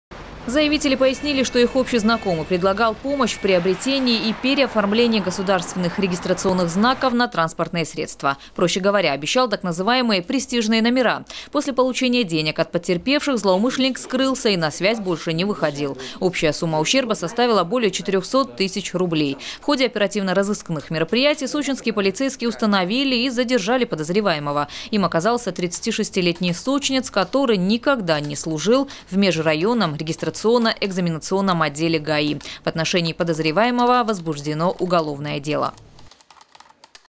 В Сочи задержан подозреваемый в совершении серии мошенничеств. Новости Сочи Эфкате
В дежурную часть отдела полиции обратились трое местных жителей и сообщили о том, что они стали жертвами злоумышленника.
Сочи. Новости.
Телеканал Эфкате.